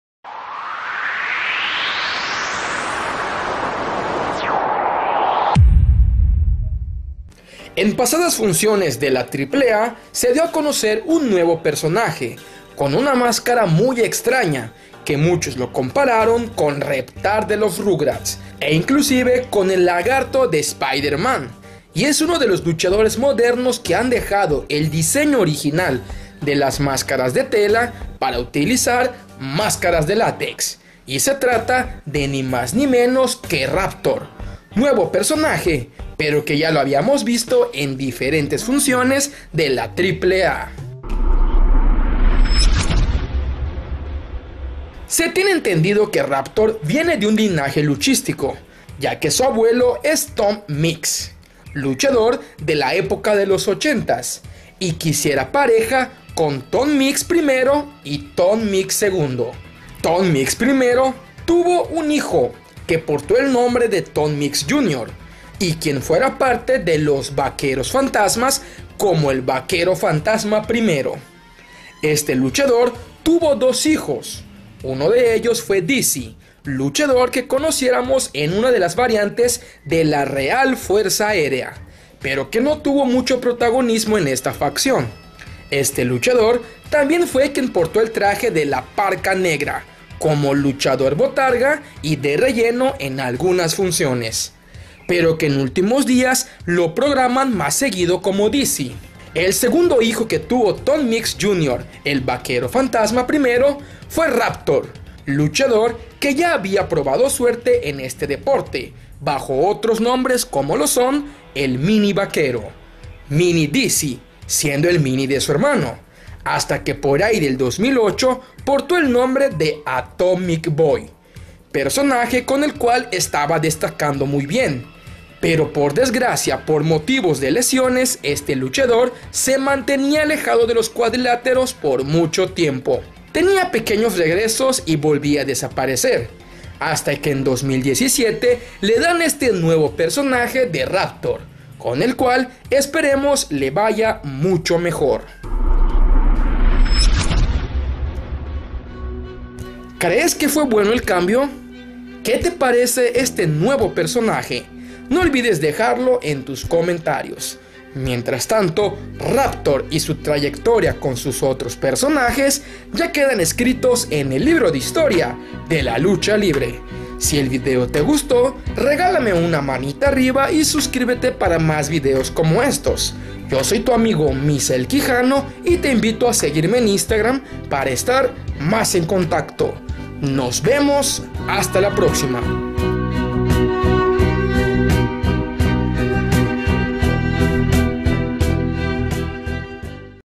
¿Quién es Raptor?
hay un personaje que esta dando de que hablar en AAA, pero la pregunta es ¿Quién es Raptor?